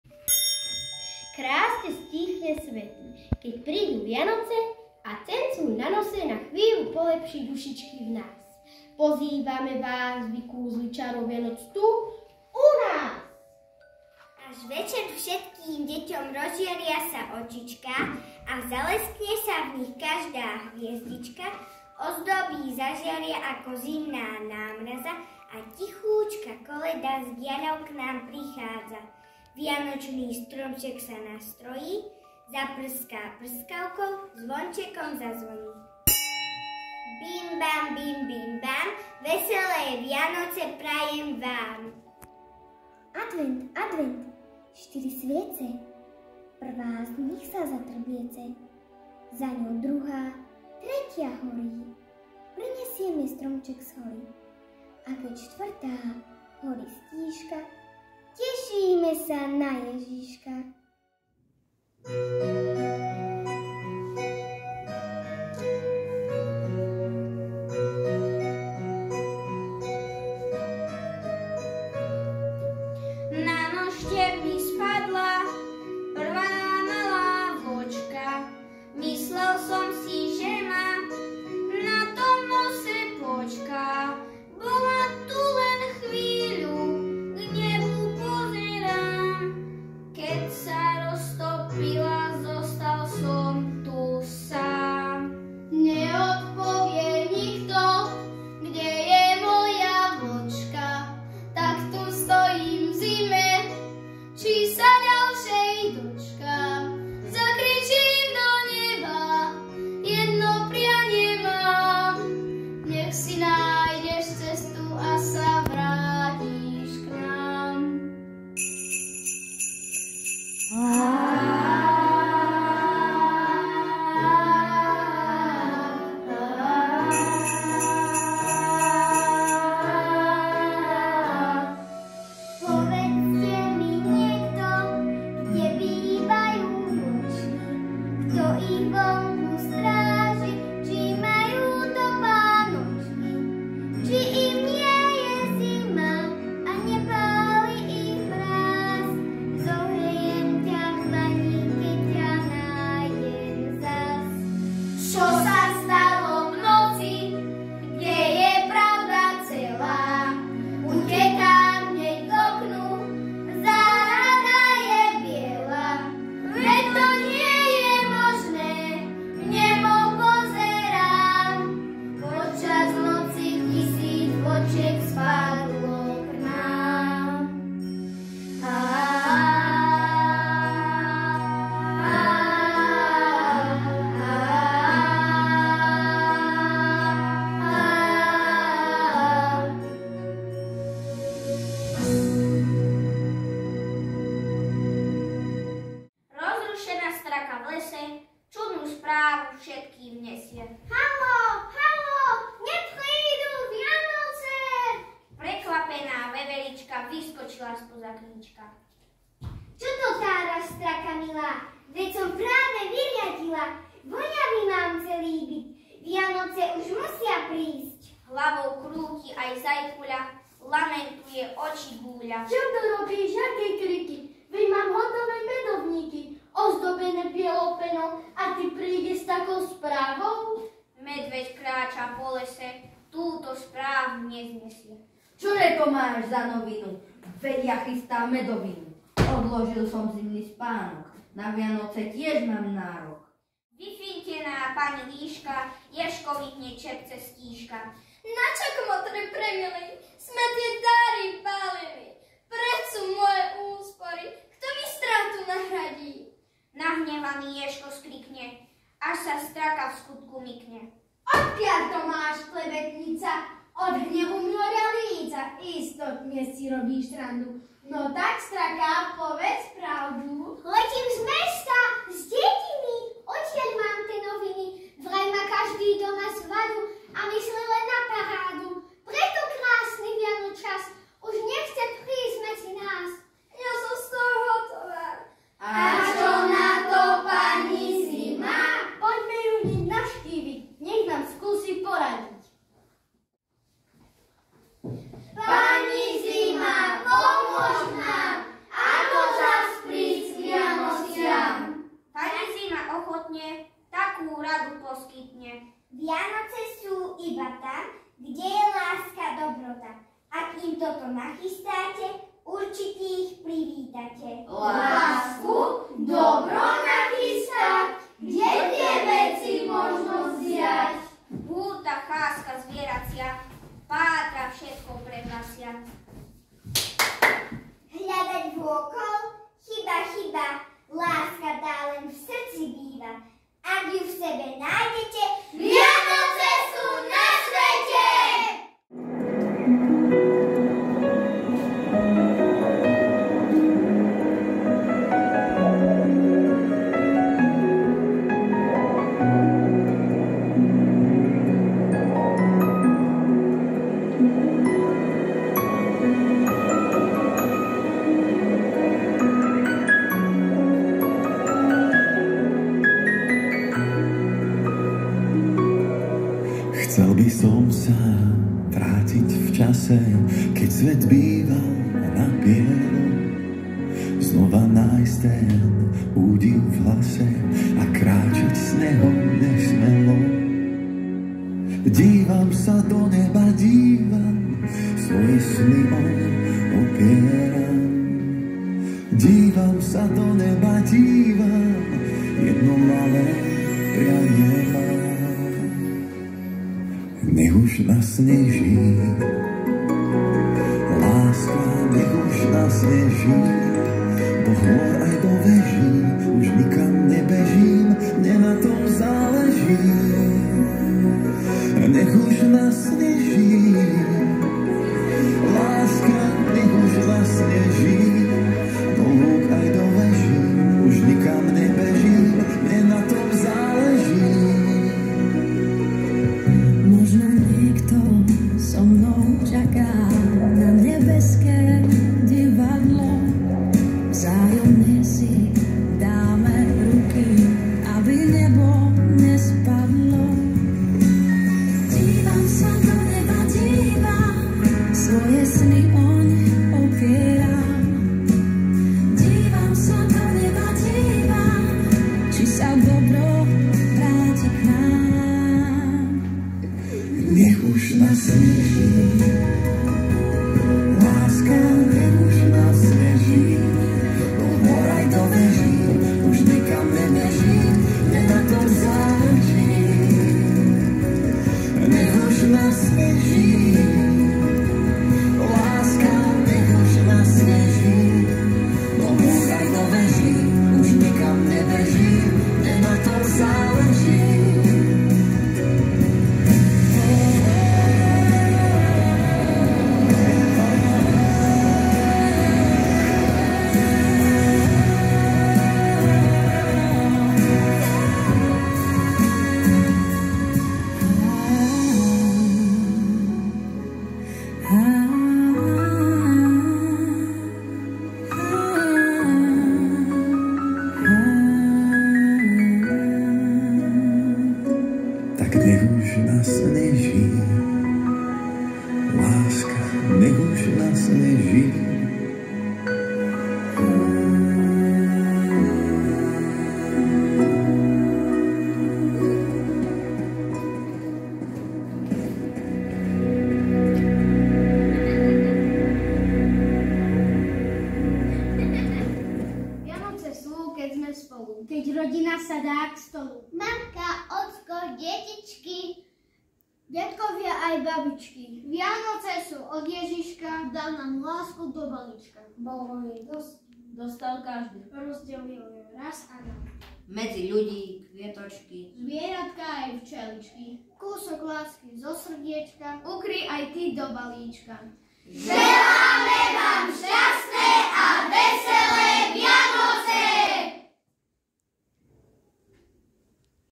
3.B NECH UŽ NASNEŽÍ! VIANOČNÝ PROGRAM 2023
Nech už nasneží!
A tento rok už aj  skutočne nasnežilo. Biele, pokojné a ničím nerušené  vianočné sviatky vám želá 3. B!